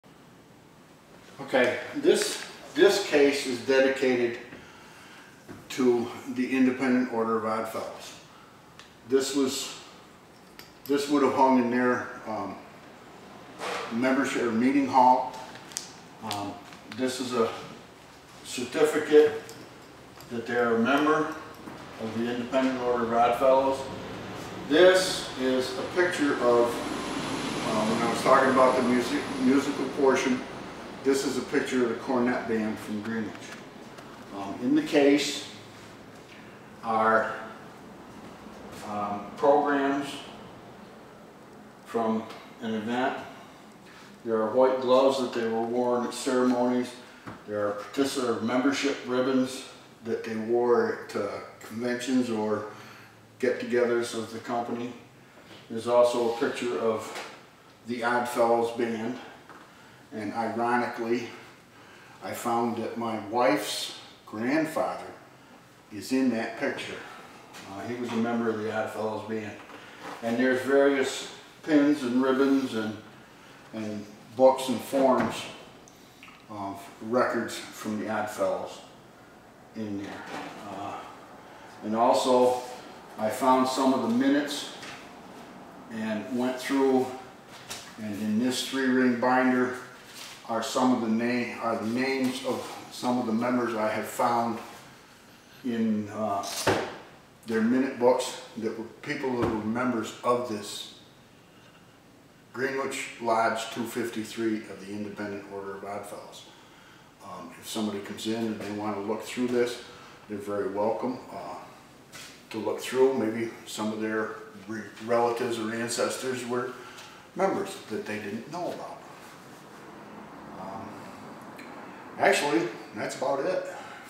Rough & Ready Virtual Tour- Odd Fellows
Gary St Mary talks about the usage of the building by the Independent Order of Odd Fellows.